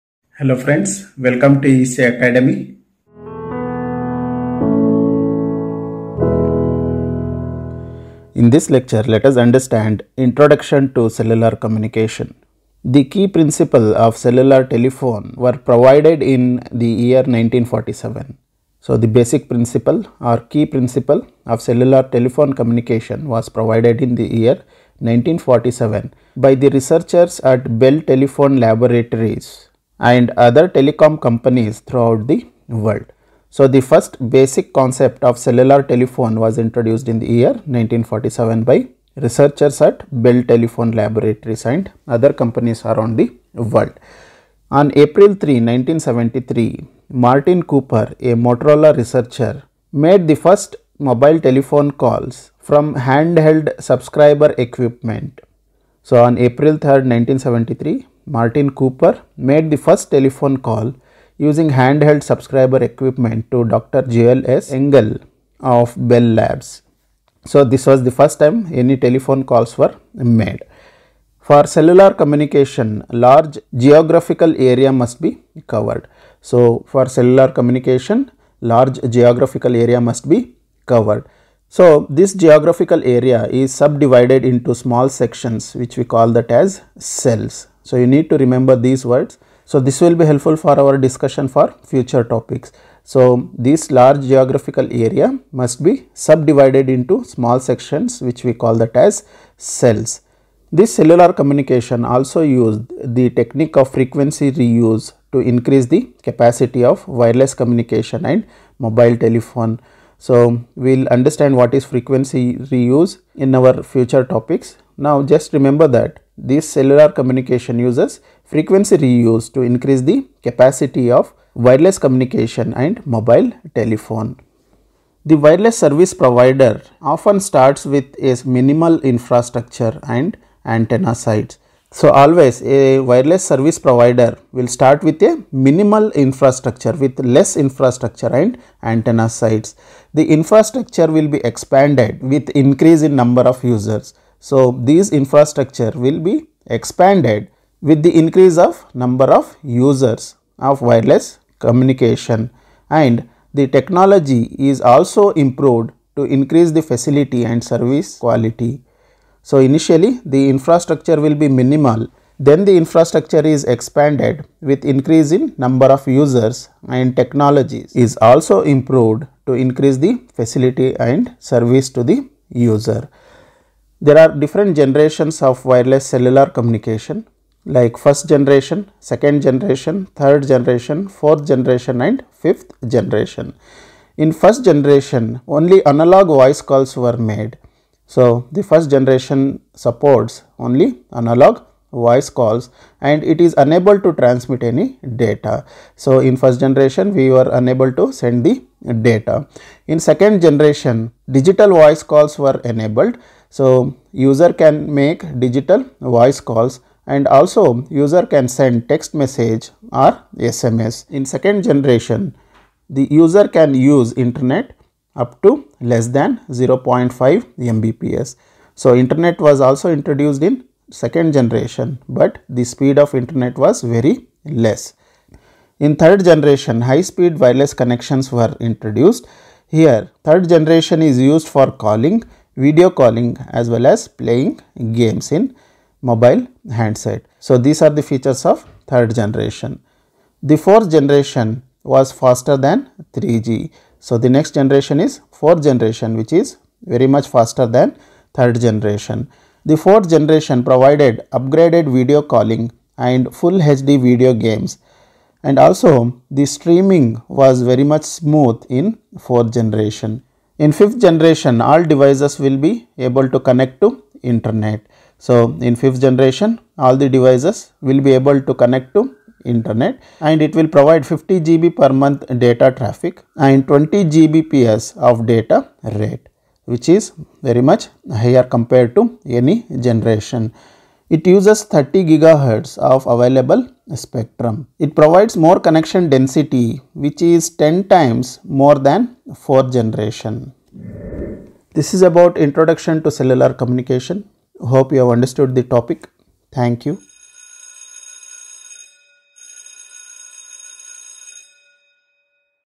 Introduction to CELLULAR COMMUNICATION SYSTEM electronics & communication engineering || EC ACADEMY
In this lecture, we will understand Introduction to CELLULAR COMMUNICATION SYSTEM in electronics & communication engineering.